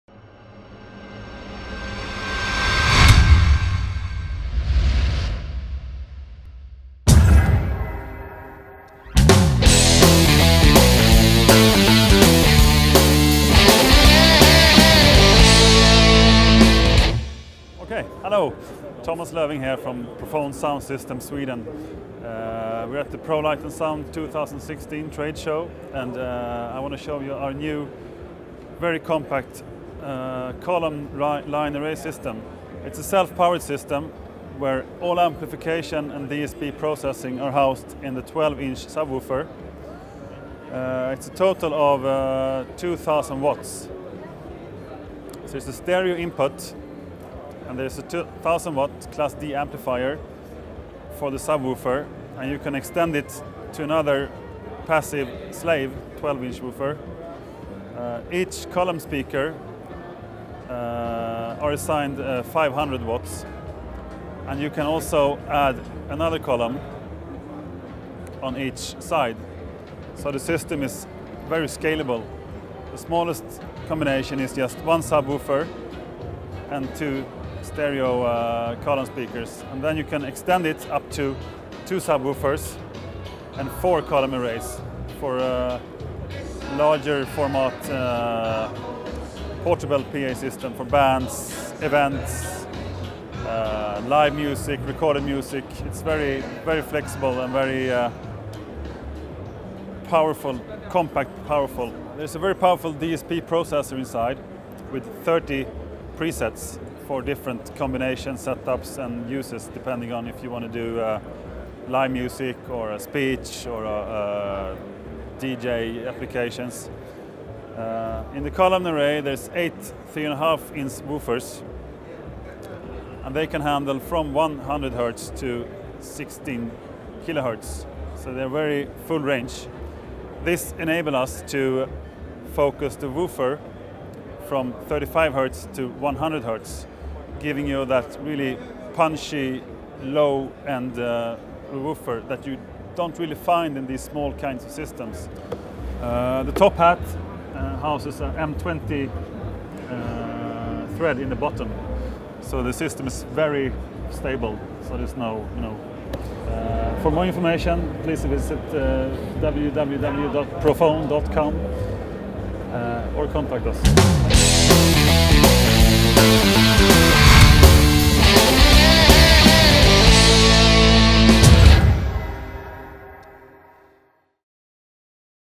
Prophon column array system A11 - Frankfurt Prolight + sound 2016 presentation
Presentation of the new prophon sound systems, compact active column array system, A11, at the 2016 Prolight+sound trade show in frankfurt.
With 12" active subwoofer, housing all electronics, amplifiers and DSP´s, and up to 4 pcs. of 8x3,5" column array speakers. a total of 2000W class-D amplifiers, with switched mode power-supply, handling 85-265V AC input.
1000W + 500W + 500W RMS stereo system.
30 Presets for a variaty of settings and setups.
Designed for bands, DJ´s, the rental industry, events and installation.